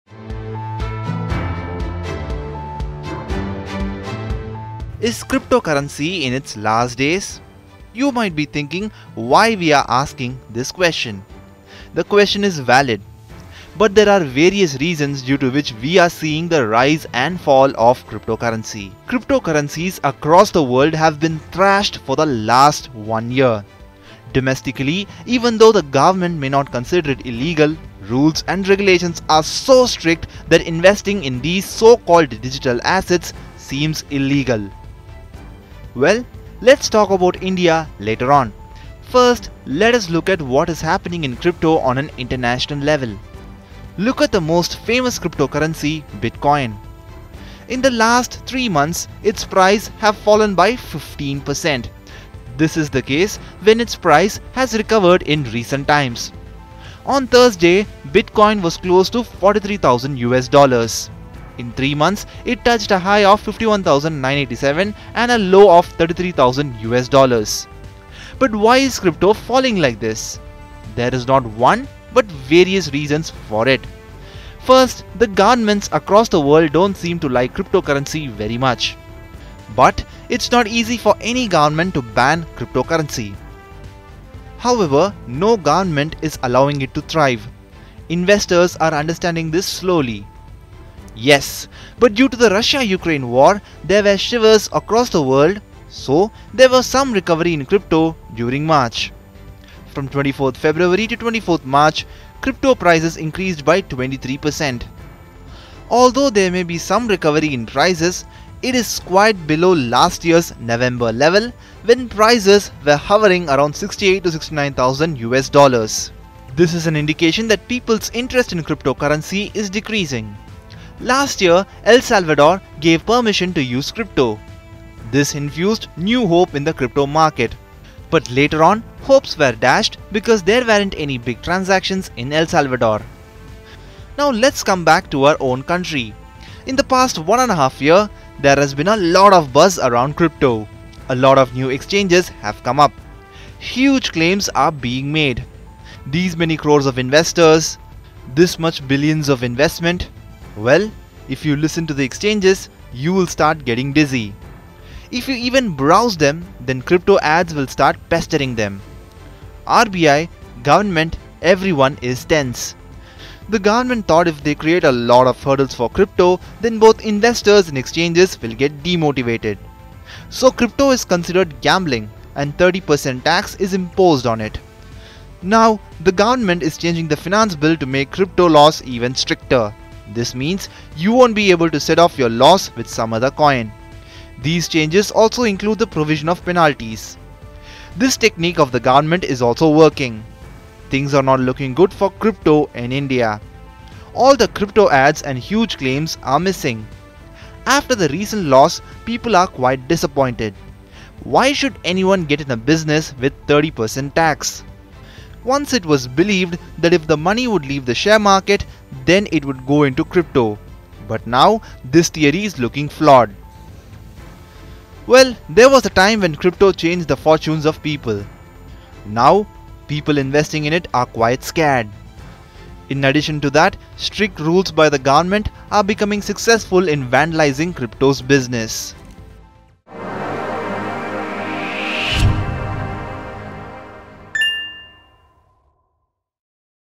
Cryptocurrency could be in jeopardy with government restrictions | Money9 English
Cryptocurrencies are not considered illegal. But governments across the world are working so hard to make it illegal by bringing in various restrictions.